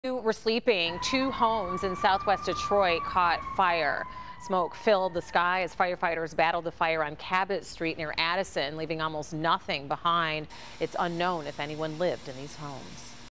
2 homes burn on Cabot Street in Southwest Detroit
Video shows smoke filled the sky as firefighters battled the flames on Cabot Street near Addison Street. The structures were almost totally burned.